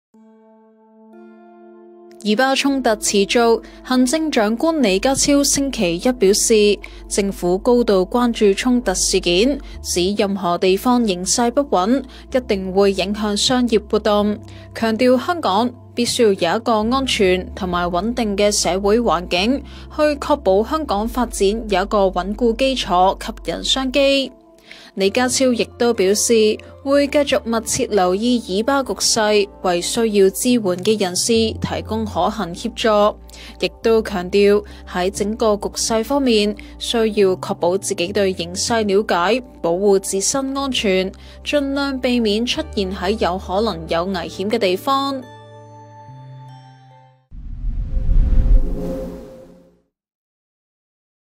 以巴衝突｜港府高度關注事件 李家超：形勢不穩影響商業活動 以巴開戰｜以色列｜巴勒斯坦｜哈馬斯｜香港｜商業｜中東｜投資
以色列與巴勒斯坦加沙地帶武裝組織哈馬斯爆發的激烈衝突持續，行政長官李家超今日（16日）見記者時被問到早前出訪中東開拓商機，現時如何評估在以色列甚至整個中東地區投資的風險。他表示政府高度關注衝突事件，指任何地方形勢不穩一定影響商業活動，強調香港必須要有一個安全及穩定的社會環境，以確保香港的發展是有一個穩固基礎，吸引商機。